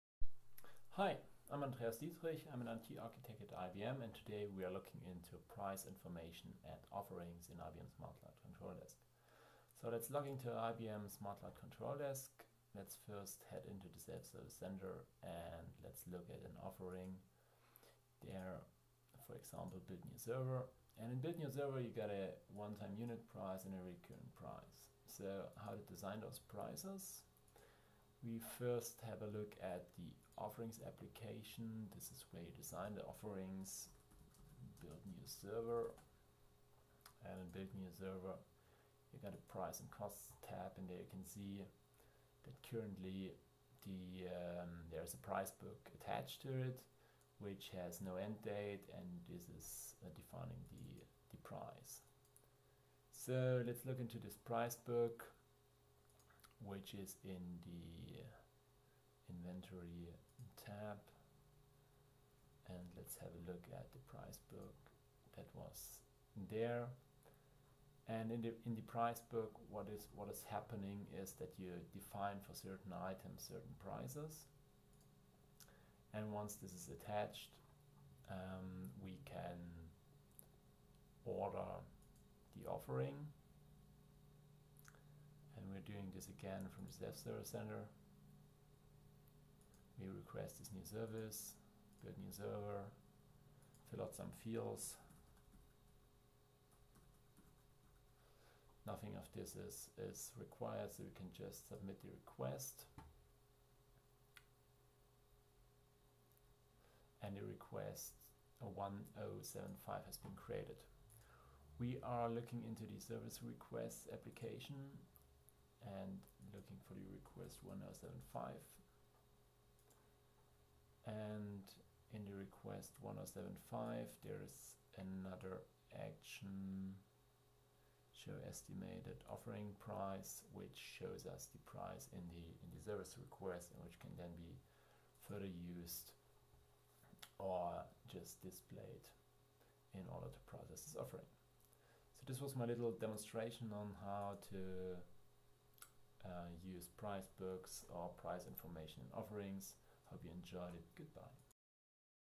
IBM SCCD Demo PriceBooks by AndreasDietrich, IT-Architect at IBM Software Services for C&SI Germany
This demo of IBM SmartCloud Control Desk 7.5.1 shows how to use PriceBooks in order to attach a price to offerings in the Self Service Center.
This price can later be seen/used in the Service Request (SR) once the offering has been ordered.